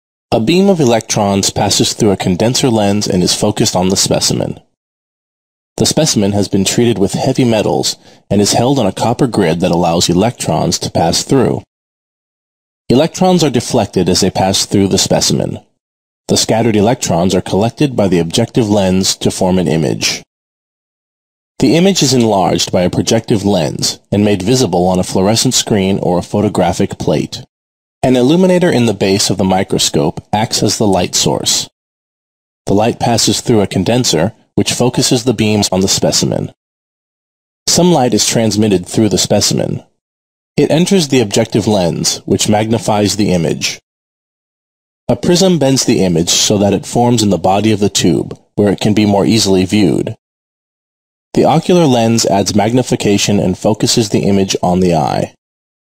How an electron and light microscopes work Animation
A beam of electrons passes through a condenser lens and is focused on the specimen.

The specimen has been treated with heavy metals and is held on a copper grid that allows electrons to pass through.

Electrons are deflected as they pass through the specimen.

The scattered electons are collected by the objective lens to form an image.

The image is enlarged by a projective lens and made visible on a fluorescent screen or a photographic plate.

An illuminator in the base of the microscope acts as the light source.

The light passes through a condenser, which focuses the beams on the specimen.

Some light is transmitted through the specimen.

It enters the objective lens, which magnifies the image.

A prism bends the image so that it forms in the body of the tube, where it can be more easily viewed.

The ocular lens adds magnification and focuses the image on the eye.